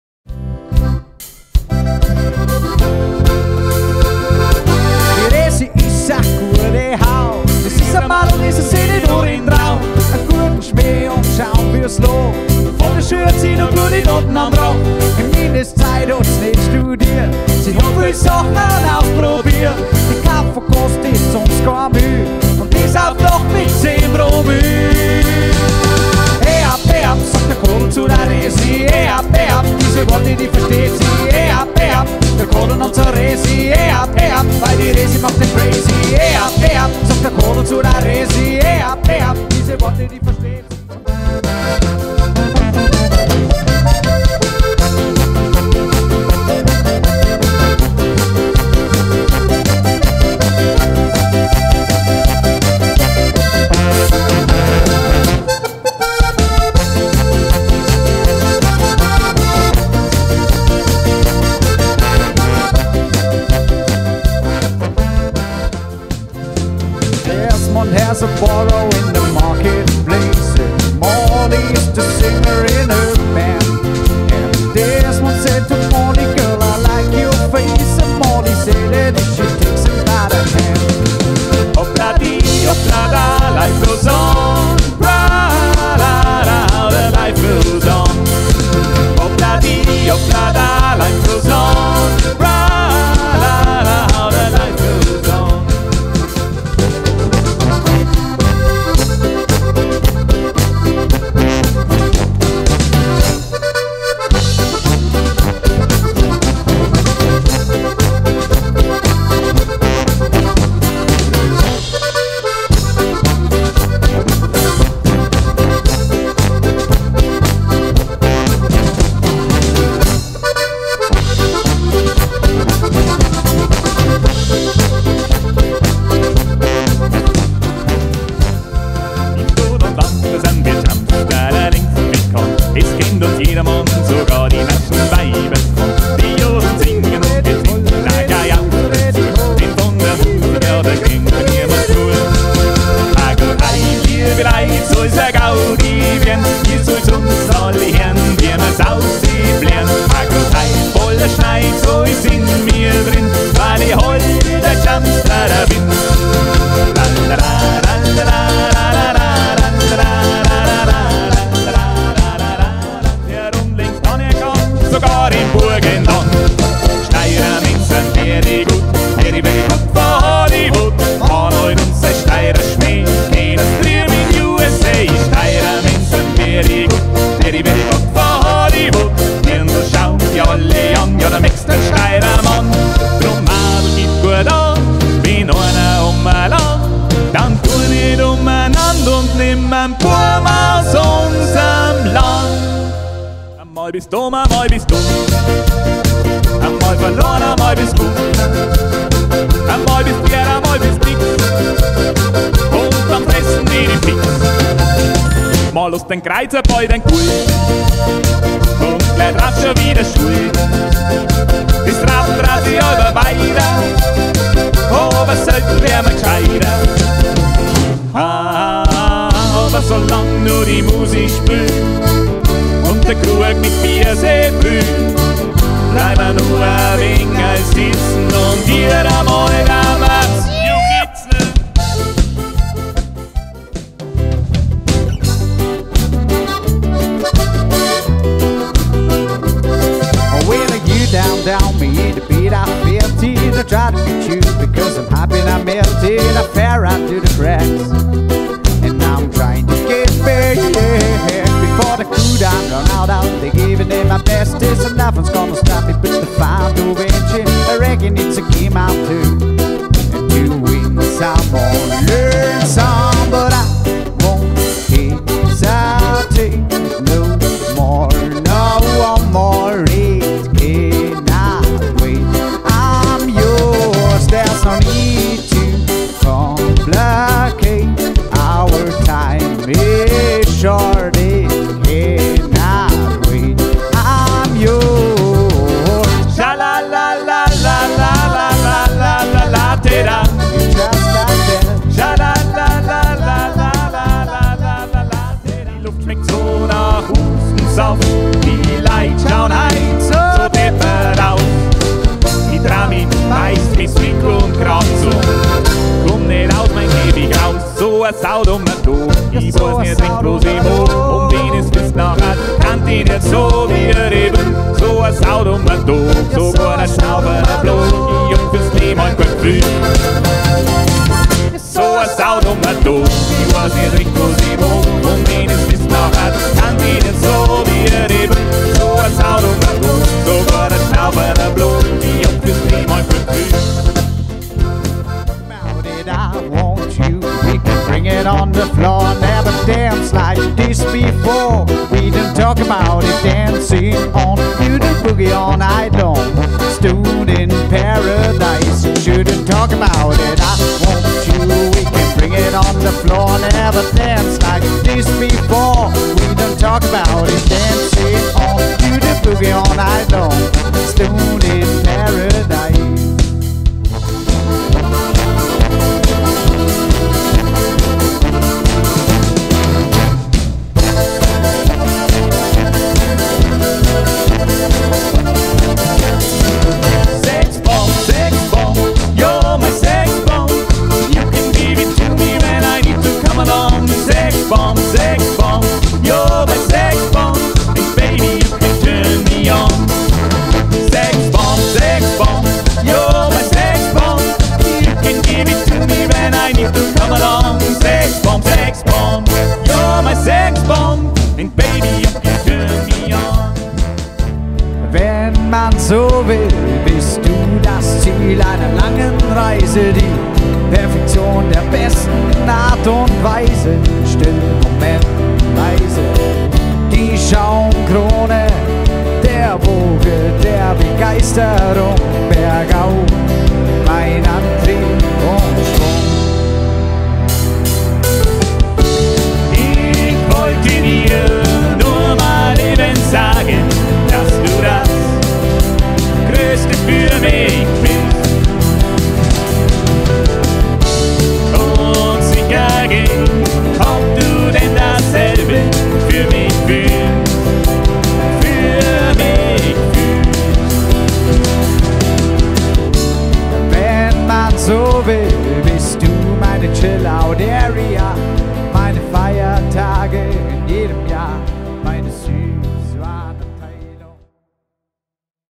Tschamstera Promotionvideo
Servus,  mir san die Tschamstera,
Marco, Luki, Marco & Johnny.
Wir spielen von zünftiger bayerischer (Tanz)- Musik bis zur Partymusik für Jung und Alt, alles was das Herz begehrt. Mit'm Timple-Boarischen, Oldies und aktueller Musik zum abfeiern sorgen wir für ausgelassene Stimmung.

Zu haben sind wir für eure Kirwa, Polterabend, Starkbierfest, Firmenfeier etc.
Meld's euch einfach, wir gfreia uns!